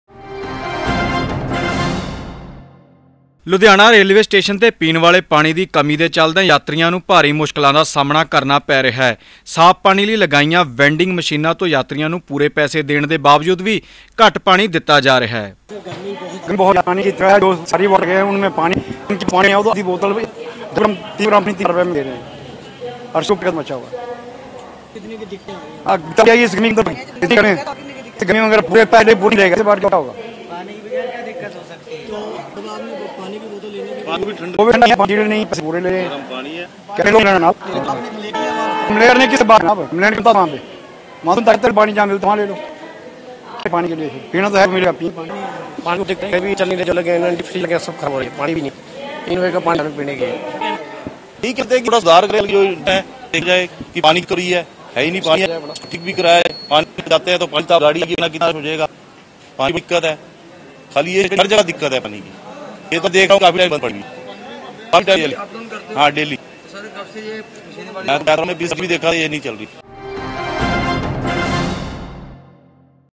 Aone Punjabi News | Ludhiana | ਰੇਲਵੇ ਸਟੇਸ਼ਨ ਤੇ ਪੀਣ ਵਾਲੇ ਪਾਣੀ ਦੀ ਹੋ ਰਹੀ ਹੈ ਕਮੀ
What'saap no.94784-84087